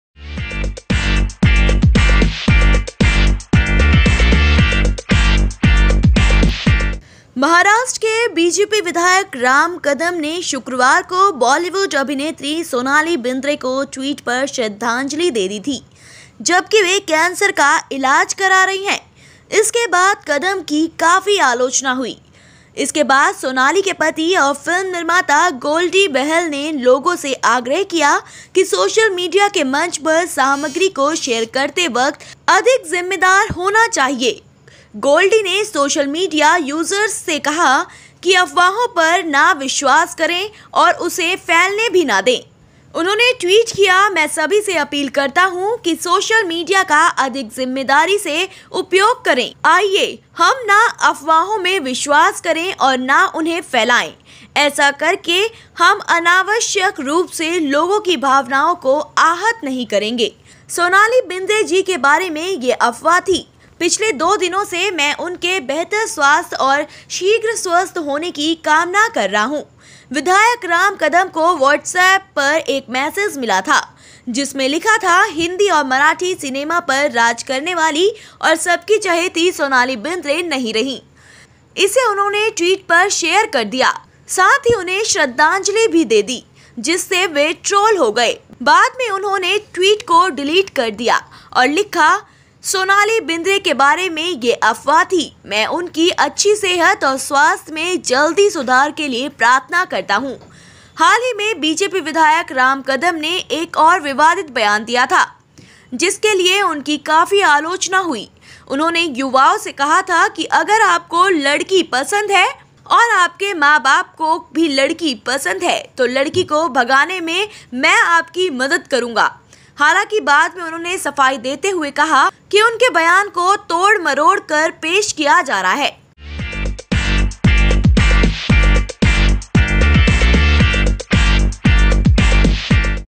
सोनाली बेंद्रे को MLA ने दी श्रद्धांजलि तो पति गोल्डी ने ऐसे किया रिएक्ट || Arrive Entertainment
महाराष्ट्र के भाजपा विधायक राम कदम ने शुक्रवार को बॉलीवुड अभिनेत्री सोनाली बेंद्रे को टि्वटर पर श्रद्धांजलि दे दी थी. जबकि वे कैंसर का इलाज करा रही हैं. इसके बाद कदम की काफी आलोचना हुई.

इसके बाद सोनाली के पति और फिल्म निर्माता गोल्डी बहल ने लोगों से आग्रह किया कि सोशल मीडिया के मंच पर सामग्री को शेयर करते वक्त अधिक जिम्मेदार होना चाहिए. गोल्डी ने सोशल मीडिया यूजर्स से कहा कि अफवाहों पर न विश्वास करें और उसे फैलाएं भी न.